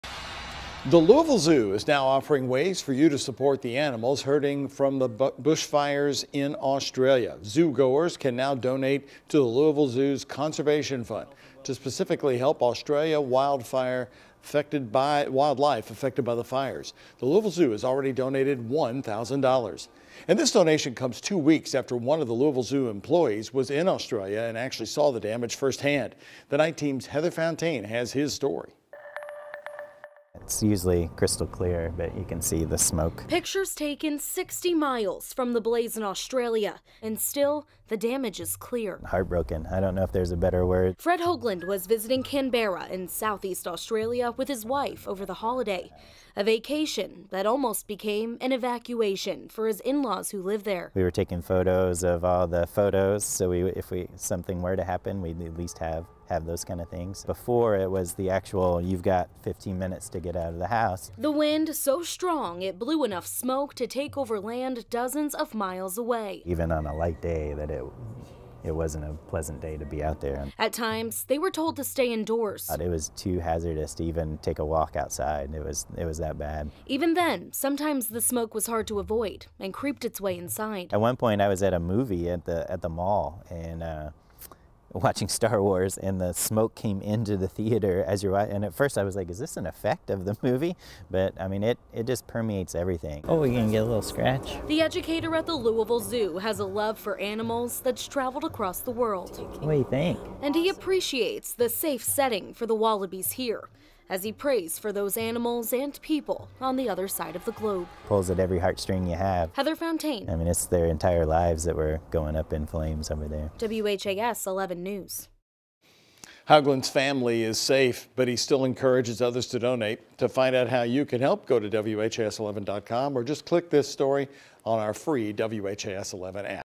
Louisville Zoo raises money for Australian animals
The Louisville Zoo is now offering ways for you to support the animals hurting from the brush fires in Australia.